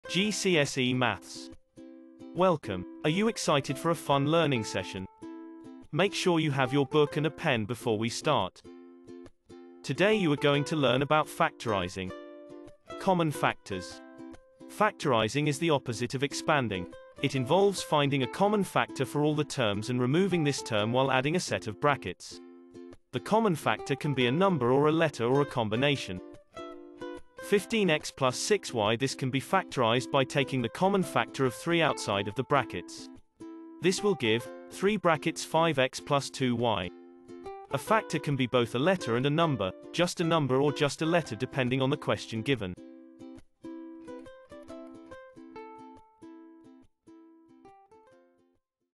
Factorising
Learn about factorising equations in this brief lesson that covers the key points you need to know for your maths GCSE